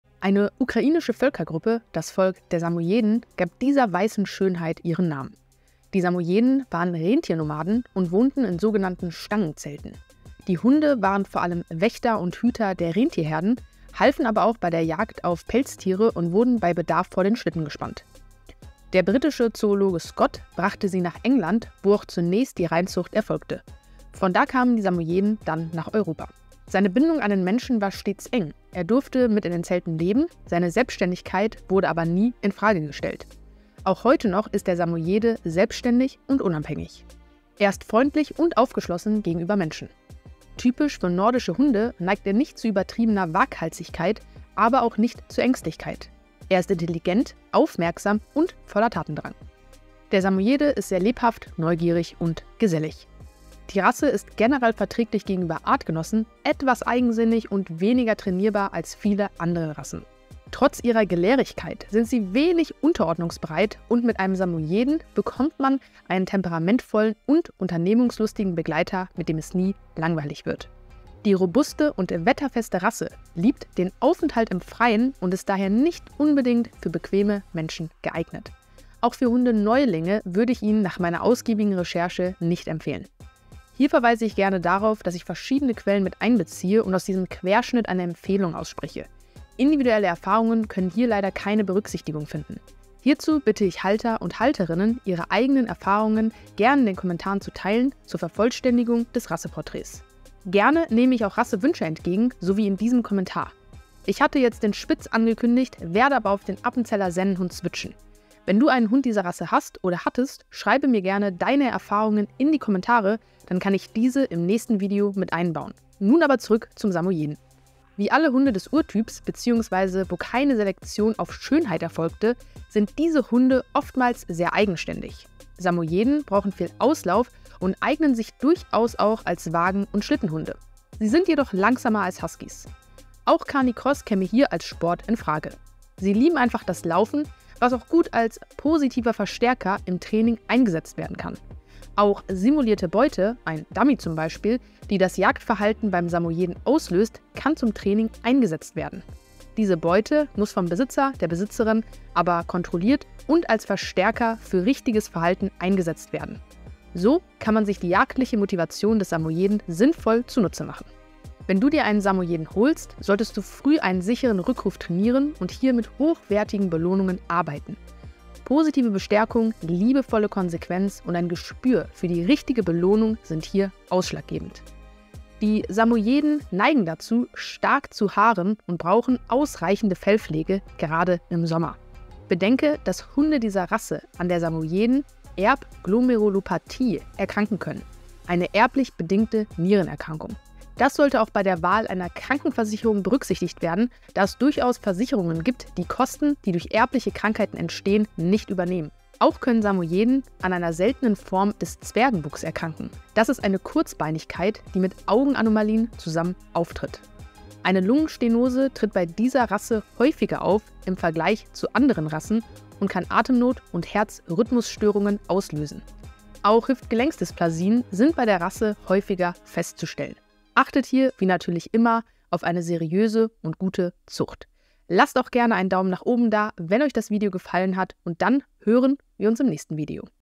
Eine weiße Schönheit - Der Samojede
Heute im Rassepotrait: Der Samojede. Ein Rasse vom Urtyp. Eine tolle Rassen, aber nicht für jeden etwas.

Noch ein paar Infos:

Lebenserwartung: 12 bis 14 Jahre
Farben: Weiß, Weiβ mit Bisquit, Creme
Temperament: Lebendig, Verspielt, Freundlich, Umgänglich, Stur, Konzentriert
Größe: Weiblich: 48–53 cm, Männlich: 53–60 cm
Gewicht: Weiblich: 16–20 kg, Männlich: 20–30 kg
Herkunft: Russland, Sibirien
Alternative Namen: Samojedenhund, Samojedenspitz

Die Kapitel

00:00 Geschichte und Ursprung
00:33 Charakter, Eigenschaften und Wesen
01:57 Training und Haltung
02:55 Pflege und Gesundheit